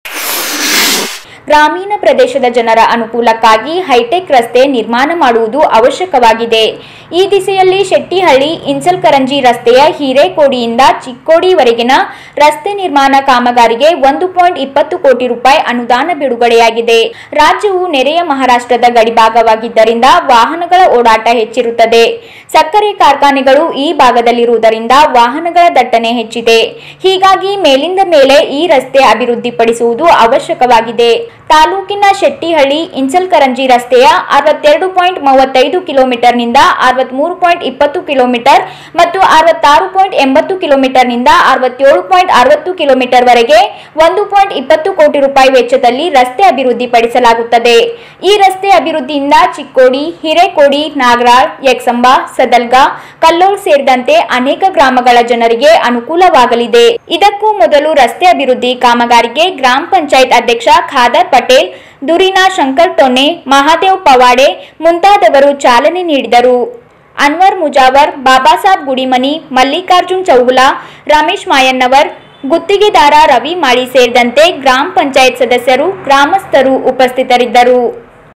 ಯಕ್ಸಂಬಾ : ರಸ್ತೆ ಕಾಮಗಾರಿ ಭೂಮಿ ಪೂಜೆ ಕಾರ್ಯಕ್ರಮ : ಗ್ರಾಮೀಣರ ಅನುಕೂಲಕ್ಕಾಗಿ ಹೈಟೆಕ್ ರಸ್ತೆ ನಿರ್ಮಾಣ
ಯಕ್ಸಂಬಾ : ರಸ್ತೆ ಕಾಮಗಾರಿ ಭೂಮಿ ಪೂಜೆ ಕಾರ್ಯಕ್ರಮ : ಗ್ರಾಮೀಣರ ಅನುಕೂಲಕ್ಕಾಗಿ ಹೈಟೆಕ್ ರಸ್ತೆ ನಿರ್ಮಾಣ (ICB NEWS EXAMBA)